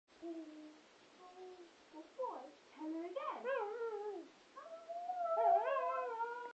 were tormenting the dog :s x
were telling the dog that were going to the park...